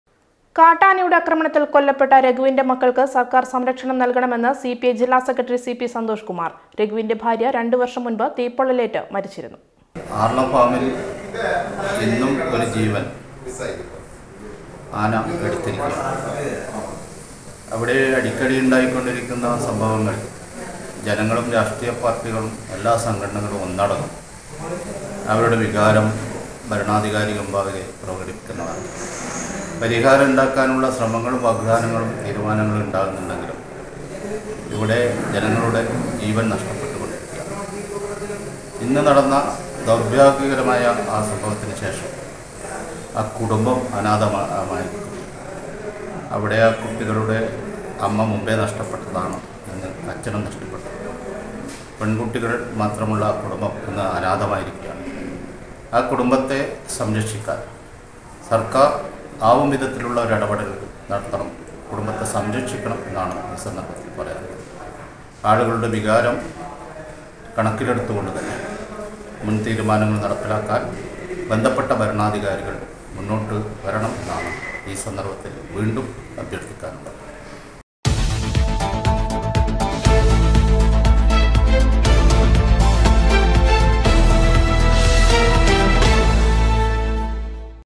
രഘുവിന്റെ മക്കള്‍ക്ക് സര്‍ക്കാര്‍ സംരക്ഷണം നല്‍കണമെന്ന് സി പി ഐ ജില്ലാ സെക്രട്ടറി
കാട്ടാനയുടെ അക്രമത്തില്‍ കൊല്ലപ്പെട്ട രഘുവിന്റെ മക്കള്‍ക്ക് സര്‍ക്കാര്‍ സംരക്ഷണം നല്‍കണമെന്ന് സി പി ഐ ജില്ലാ സെക്രട്ടറി  സി.പി സന്തോഷ്‌കുമാര്‍